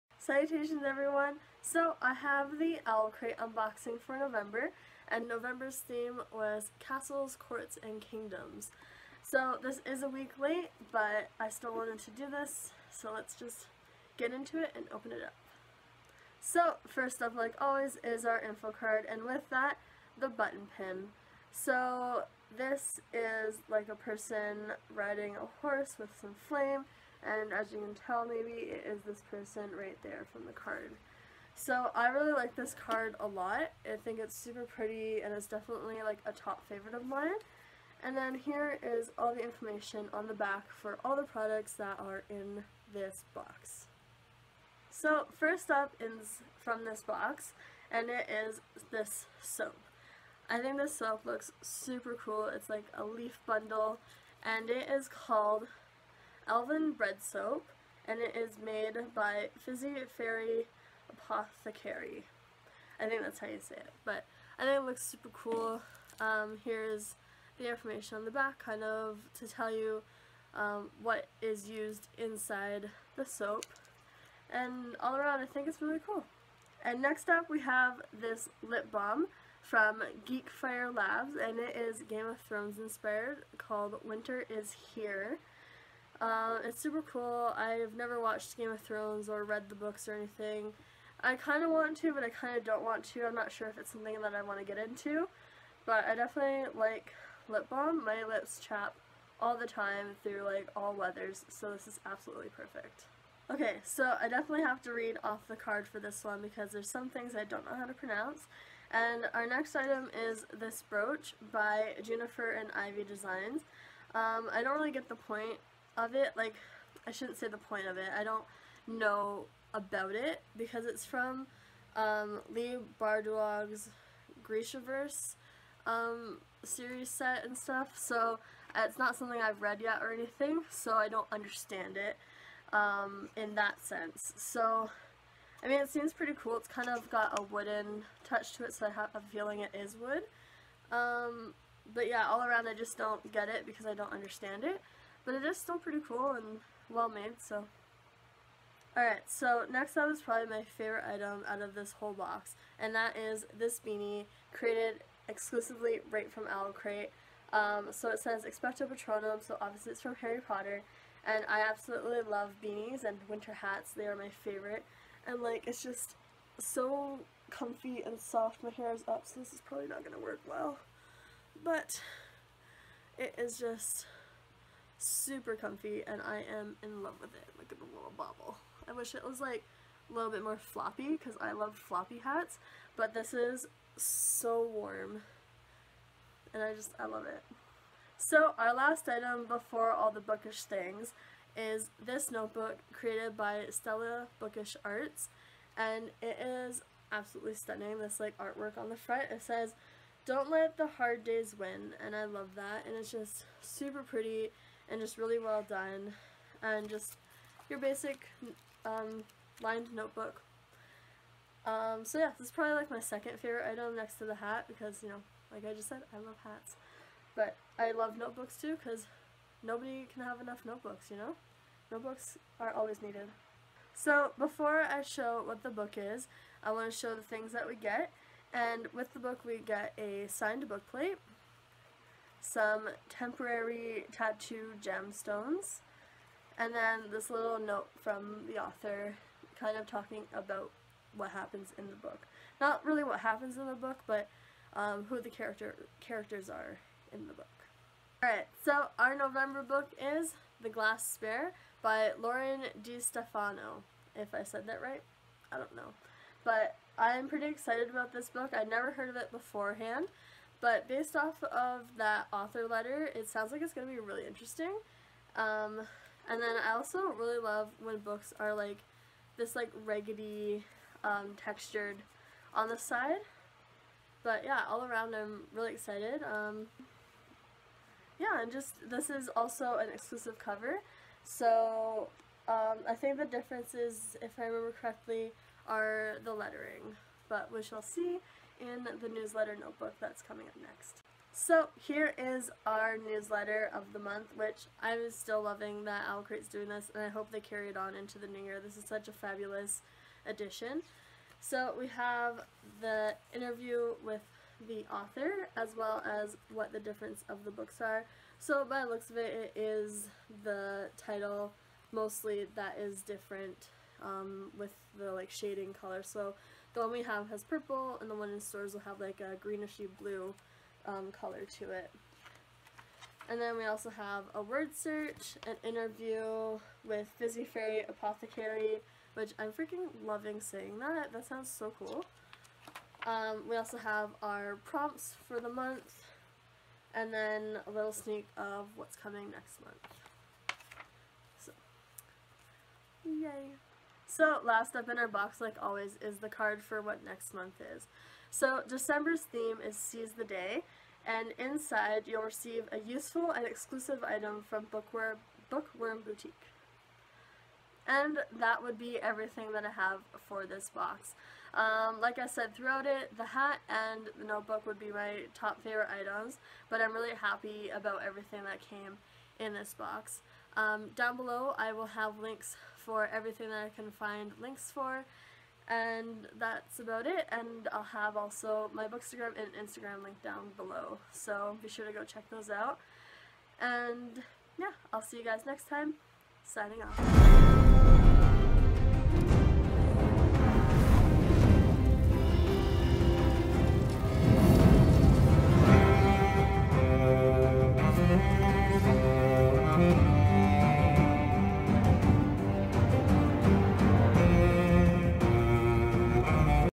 OwlCrate Unboxing: Castles, Courts, & Kingdoms - November 2017 *Review Friday
Welcome to Review Friday!

This was a pretty awesome box all around. Everything in it is super useful! I'm extremely excited about a new winter hat created by OwlCrate themselves, and the notebook. One can never have enough notebooks! xD